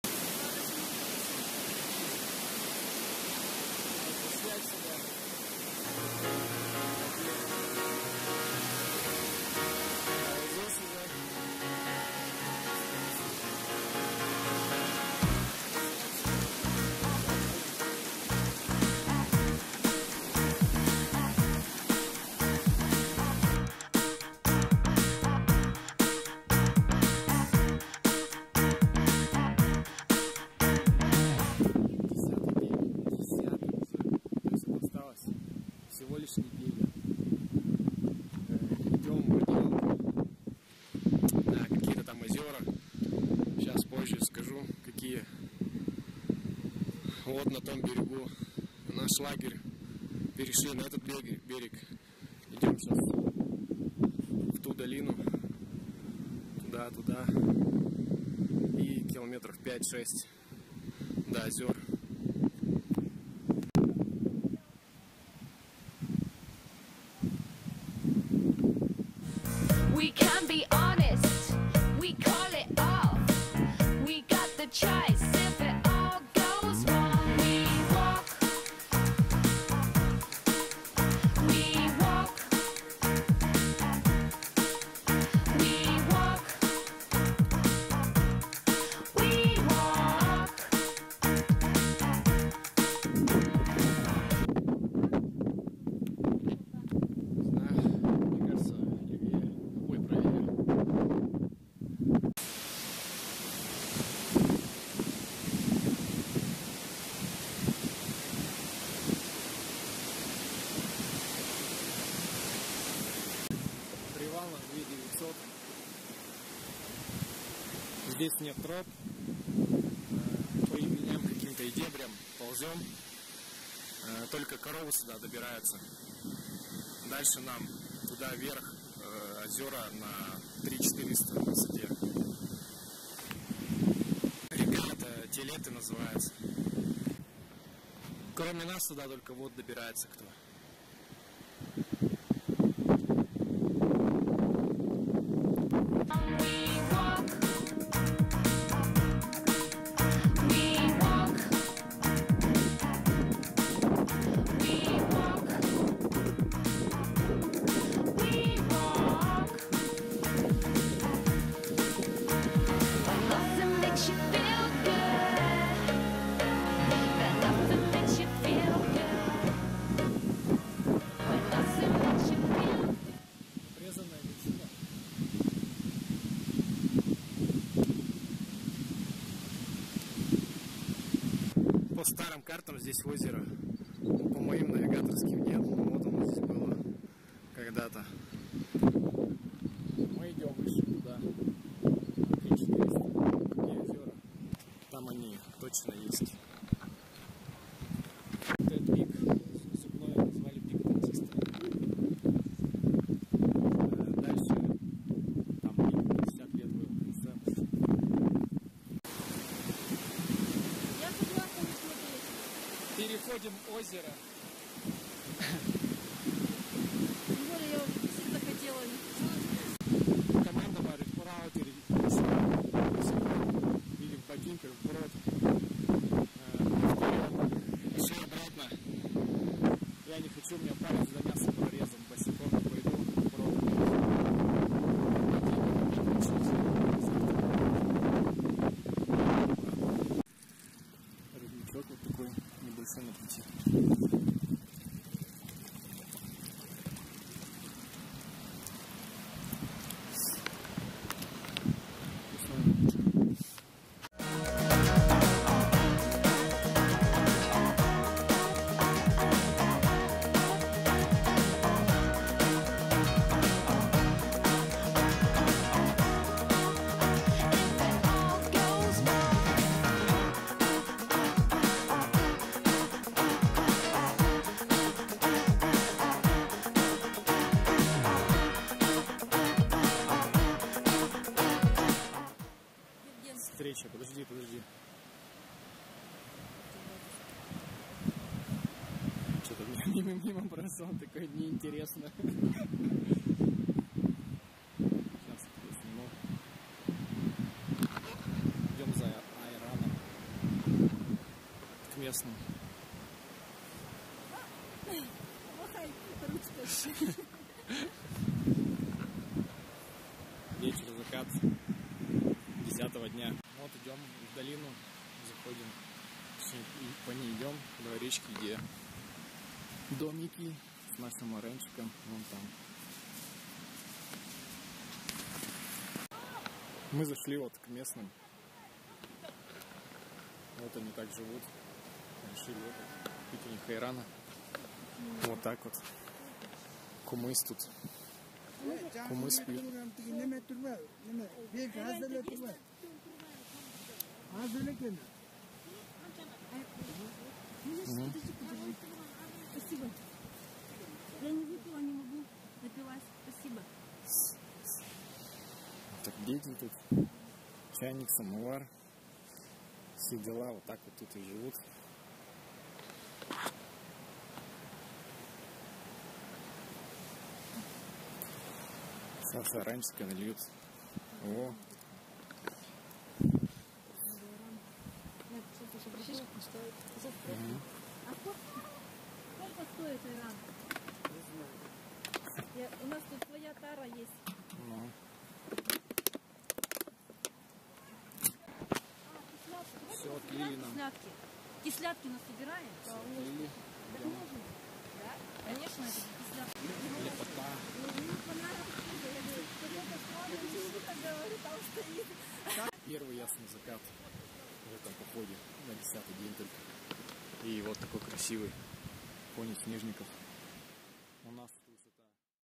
День 10. Радиалка на "озера" и за айраном. Киргизия.Тянь-Шань. Терскей-Алатау. 2018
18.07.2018. Радиалка на "озера" и потом за Айраном. Горный поход 2 к.с. по Терскей Алатау. Центральный Тянь-Шань. Киргизия.